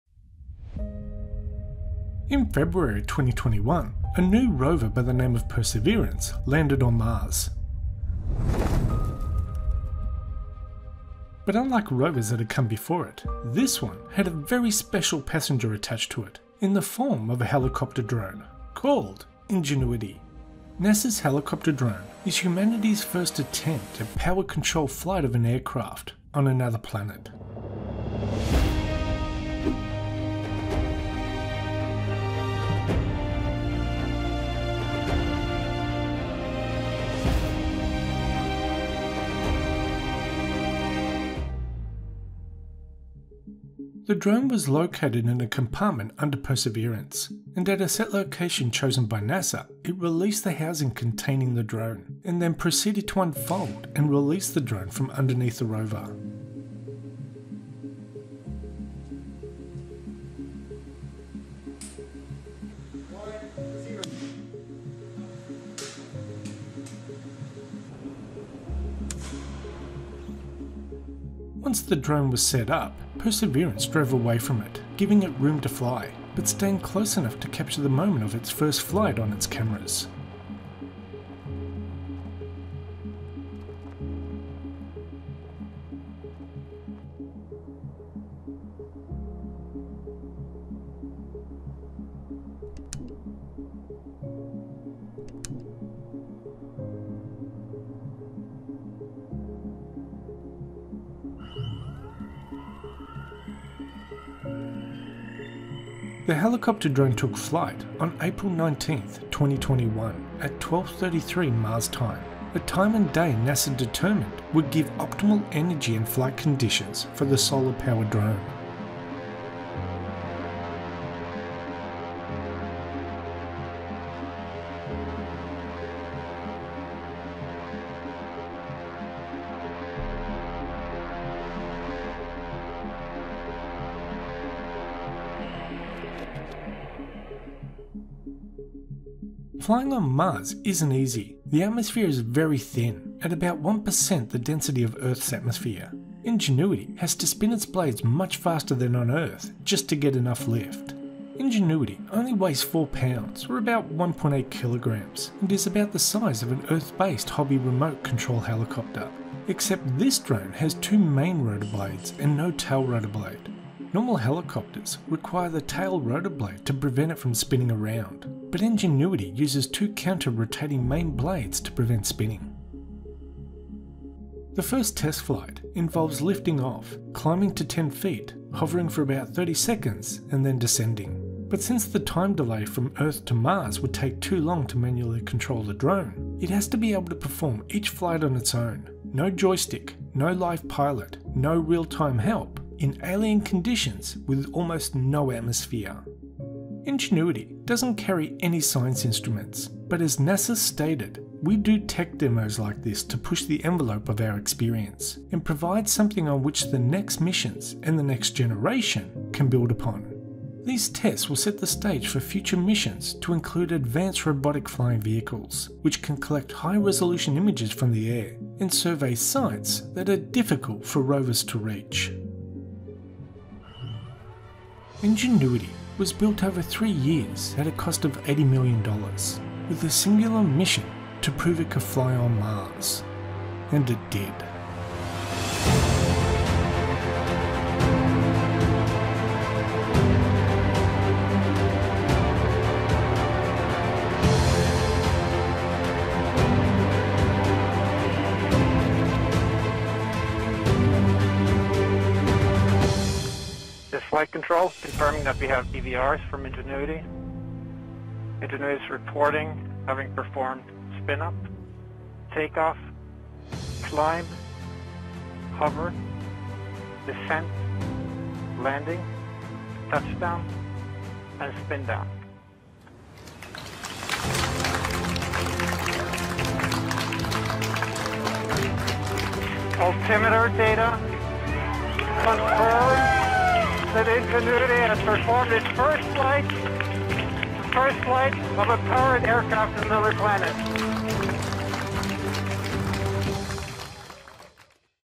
NASA Helicopter Drone Flies on Mars
In Febuary 2021, a new Rover by the name of Perseverance landed on Mars, but unlike rovers that have come before it, this one had a very special passenger attached to it, in the form of a helicopter drone, called Ingenuity.

NASA’s Helicopter drone, is humanity’s first attempt at powered controlled flight of an aircraft, on another planet.

The helicopter took flight on April 19th 2021, at 12:33 Mars Time, a time and day NASA determined would give optimal energy and flight conditions for the solar powered drone.

The first test flight involves lifting off, climbing to 10 feet, hovering for about 30 seconds, and then descending.

Since the time delay from Mission control back on earth to Mars would take too long to manually control the drone, it has to be able to perform each flight all on its own, No joystick, No live pilot, No real-time help, In alien conditions, with almost no atmosphere.

CREDIT LINKS